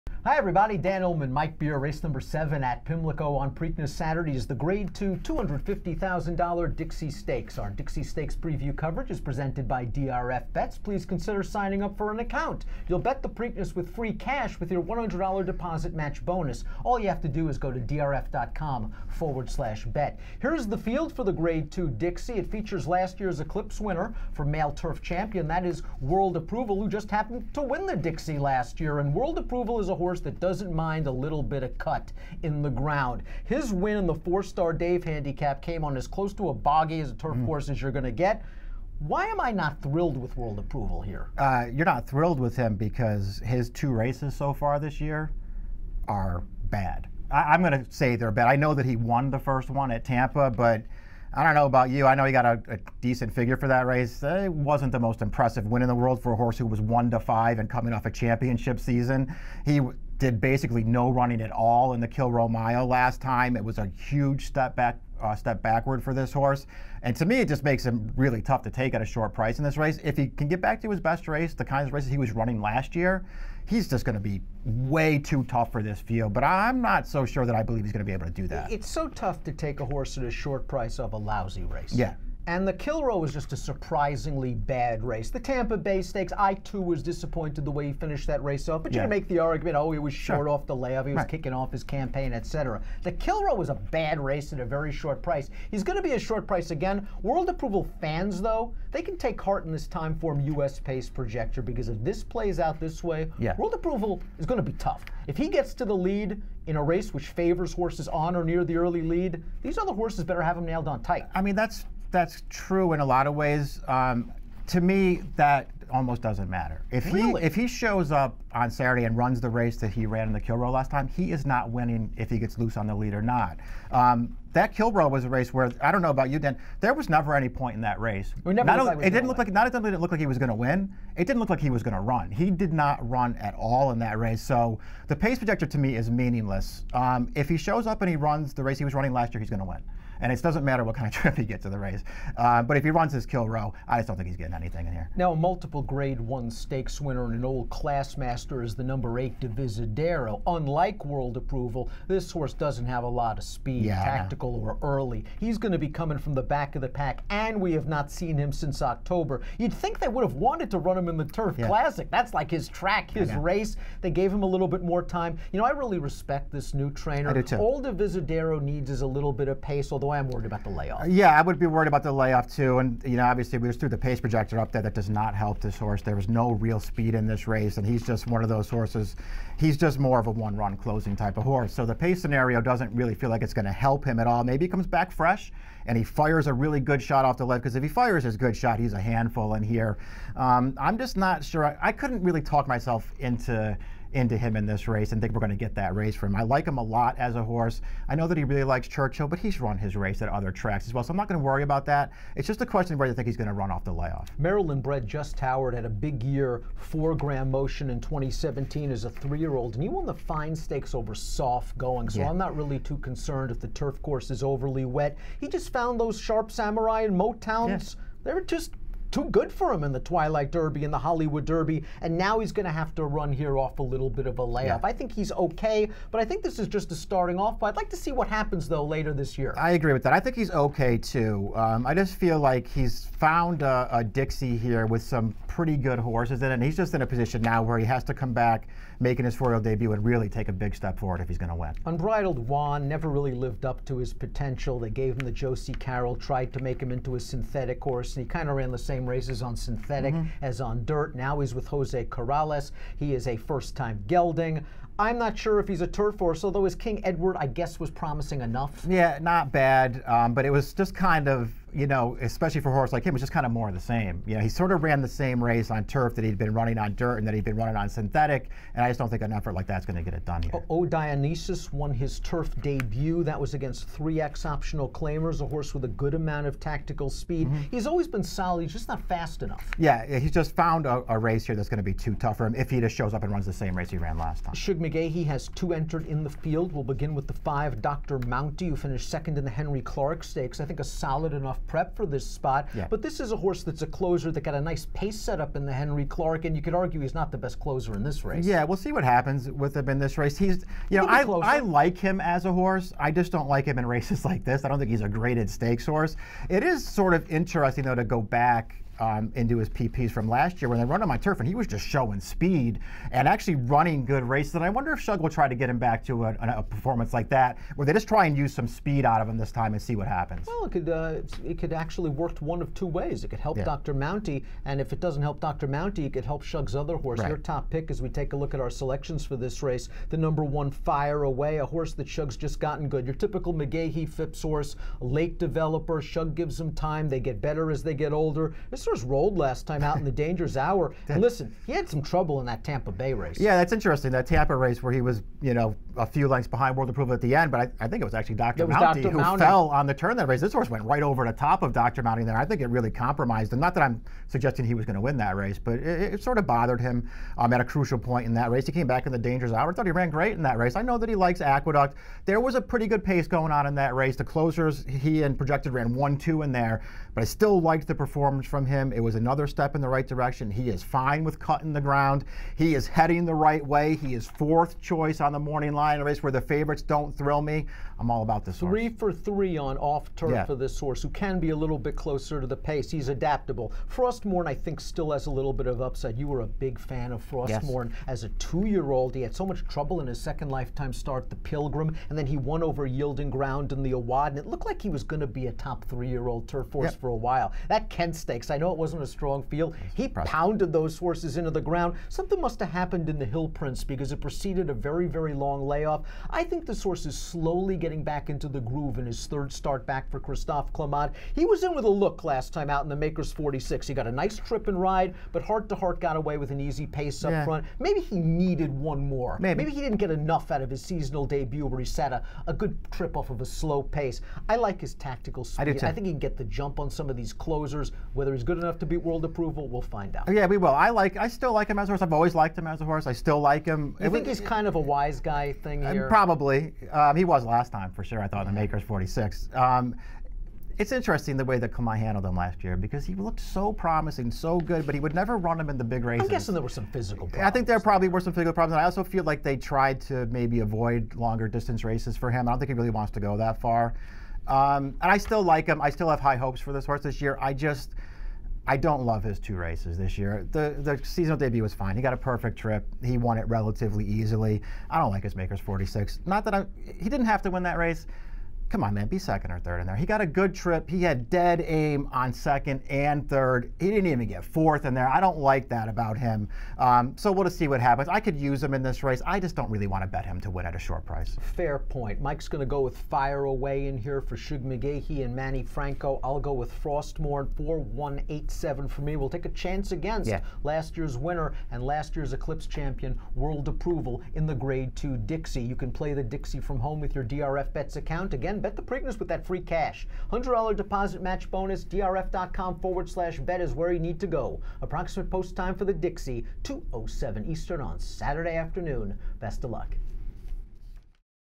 Dixie Stakes 2018 Preview
World Approval hopes to rebound from a disappointing performance when he starts favored in Saturday's Grade 2 Dixie Stakes at Pimlico. Here's Dan Illman and Mike Beer with the preview on DRFTV